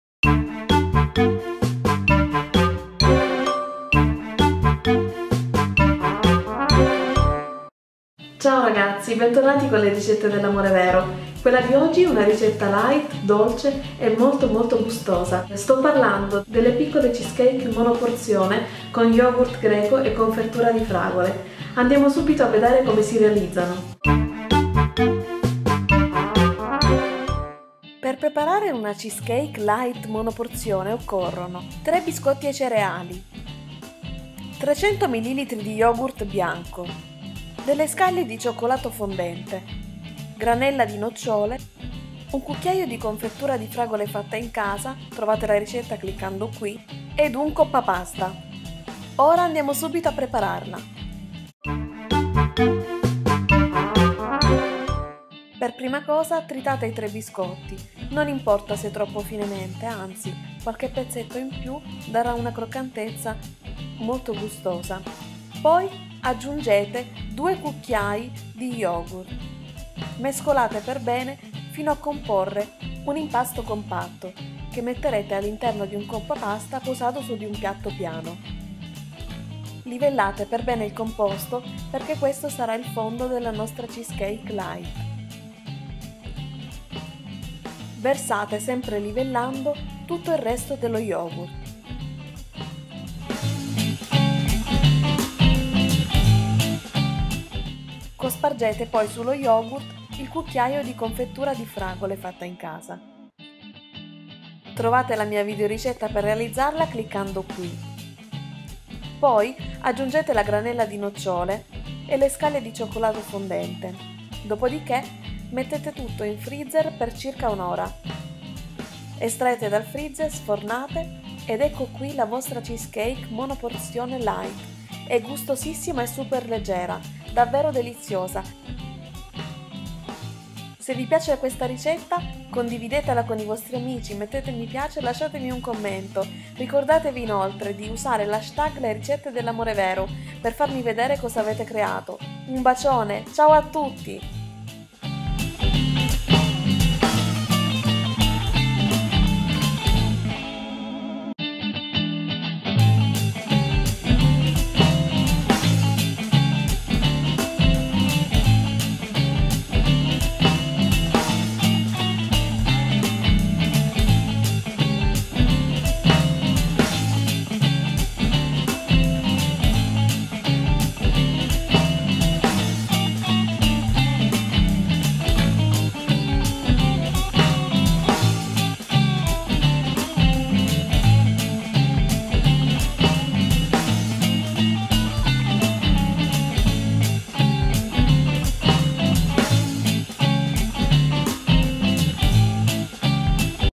CHEESECAKE LIGHT allo Yogurt e fragole, SENZA COTTURA [sub eng]
Golossima Cheesecake Light con Yogurt con fragole, nocciole e scaglie di cioccolato, perfetta per chi è a dieta, ma non vuol rinunciare al dolce!
Provate la ricetta e scrivetemi dei commenti per dirmi cosa ne pensate! :)

Per saperne di più sulla Cheesecake light vi consiglio di leggere questo articolo:

Le Ricette dell'Amore Vero di Claudia Annie:
videoricette di cucina, cosmetici fai da te, consigli utili, tour e chiacchiere.

♥♥♥ RUBRICHE PER VOI ♥♥♥